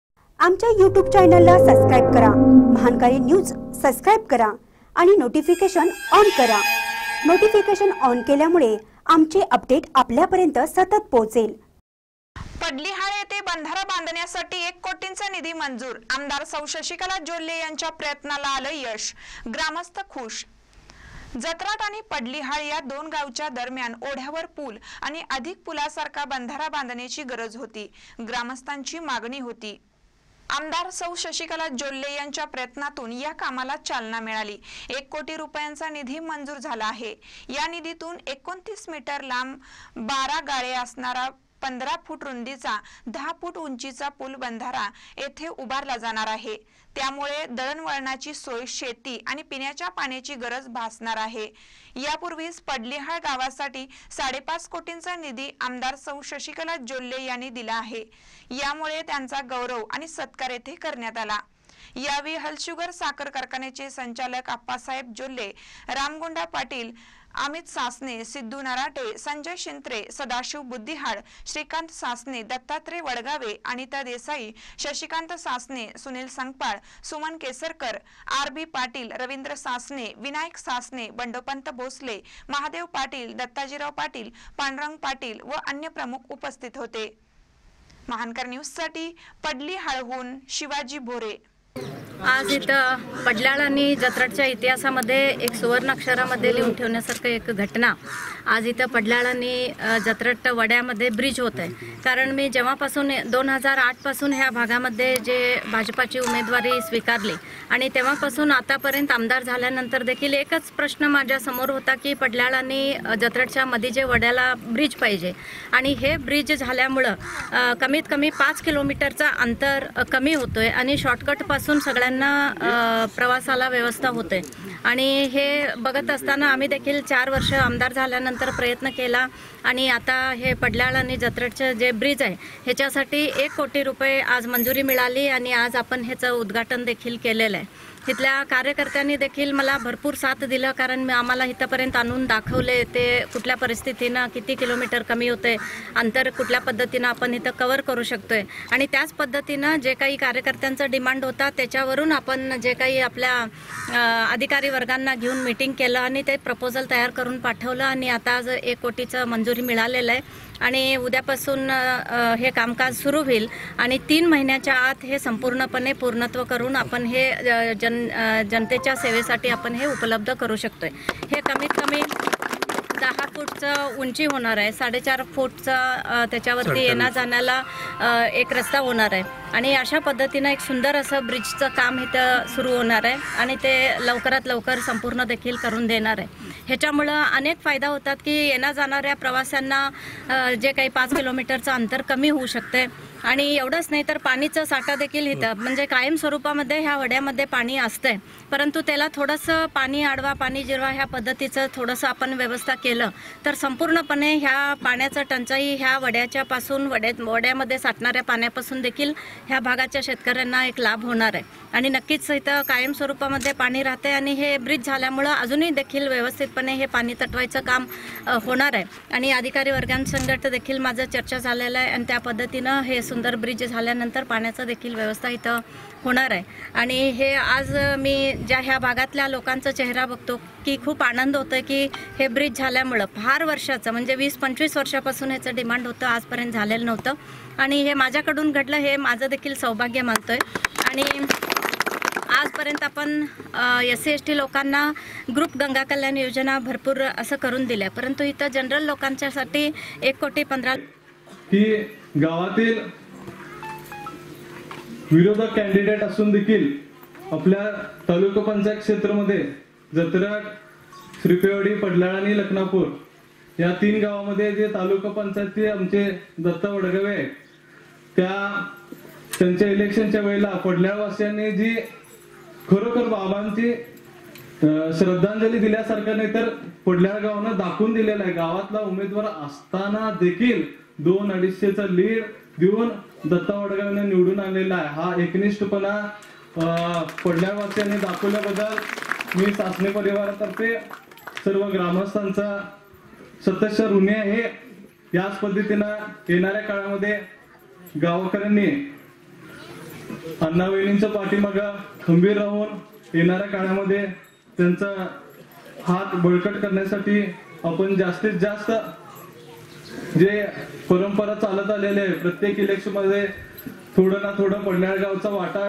पडलीहाळ येथे बंधारा बांधण्यासाठी एक कोटीचा निधी मंजूर
पडलीहाळ येथे बंधारा बांधण्यासाठी एक कोटीचा निधी मंजूर आम. सौ. शशिकला जोल्ले यांच्या प्रयत्नाला आलं यश ग्रामस्थ खूश